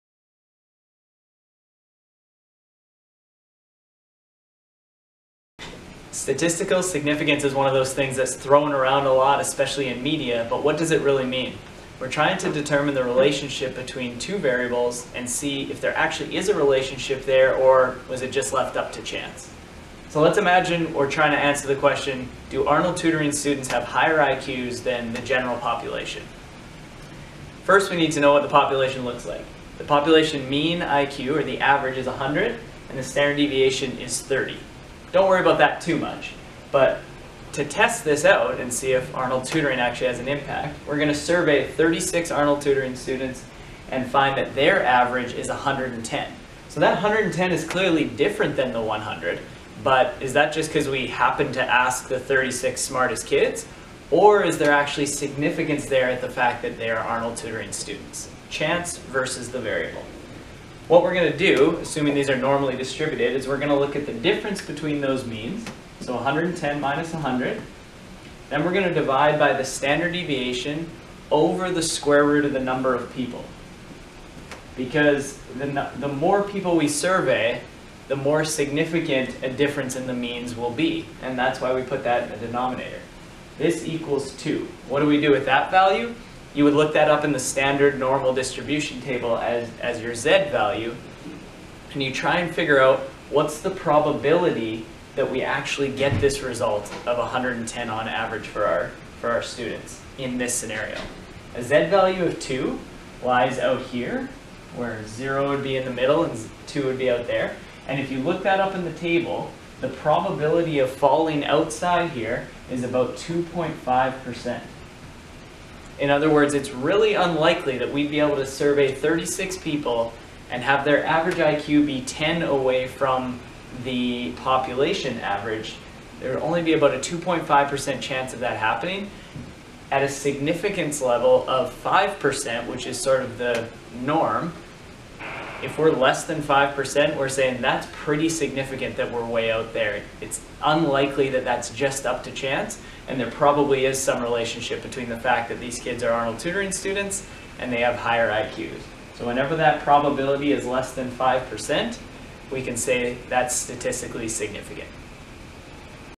Statistical Significance
We hear the term alot, but what does statistical significance even mean? We are trying to determine whether there is a relationship between two variables (in this case where they get tutoring, and student IQ) that is more than chance.
Any time you collect data, there's an element of random chance that you selected the smartest, tallest, or fastest people. However, if the results are so far away from expected, there's a smaller and smaller likelihood that it was up to random chance, deeming it statistically significant.